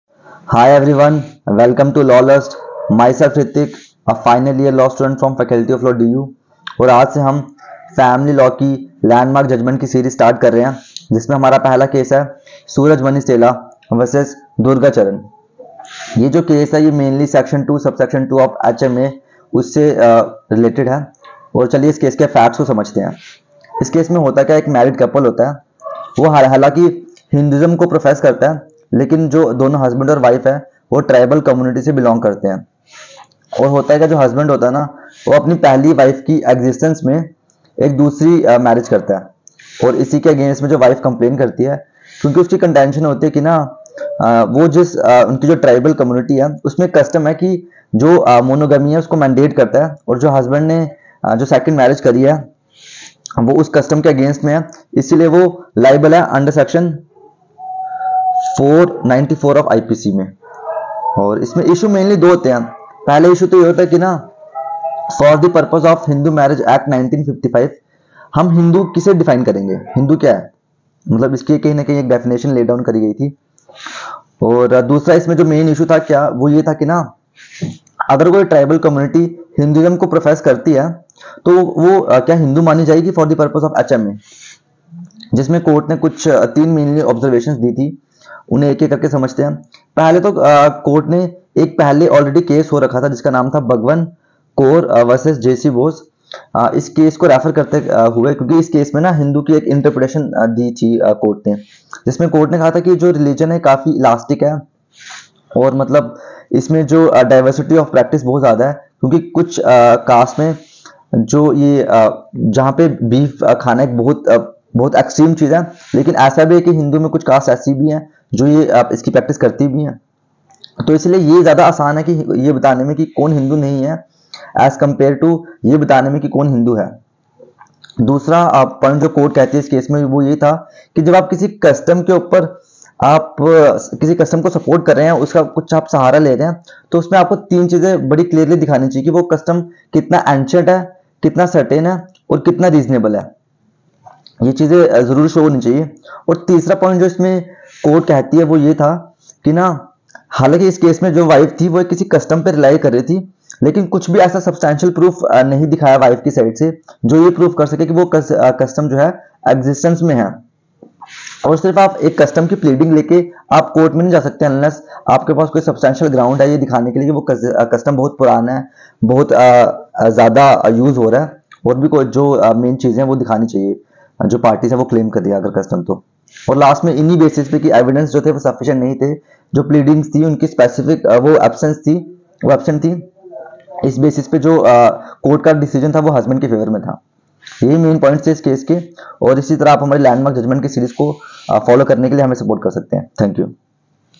Surajmani Stella Kujur v. Durga Charan Hansdah | Family Law Case Series | Episode 1:familylaw
Description:
🔍 Dive into the intricate world of family law with our new video series! 🌐 In the inaugural episode, we dissect the landmark case of Surajmani Stella Kujur v. Durga Charan Hansdah, shedding light on pivotal legal aspects that have shaped family law jurisprudence.

📚 In this comprehensive analysis, we explore the facts, legal arguments, and the groundbreaking judgment that emerged from this case. From complex custody issues to the evolving nature of marital rights, we unravel the legal intricacies that continue to influence family law cases today.

lawlust

Thank you for joining us on this legal journey! 📜🔒